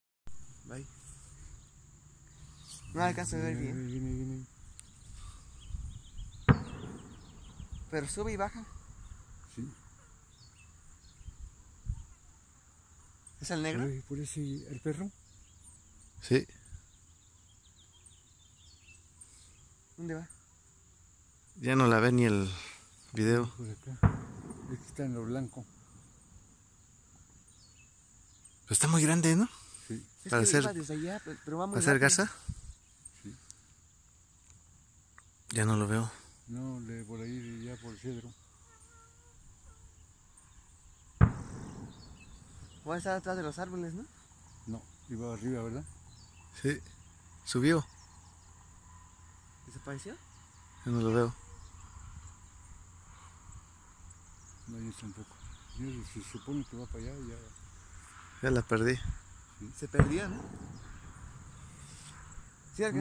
Ovni en tlalmanalco,Estado de México . Parte 2
Video de ovni captado en el municipio de tlalmanalco Estado de México. Fue ubicado en el pueblo de San Juan Atzacualoya .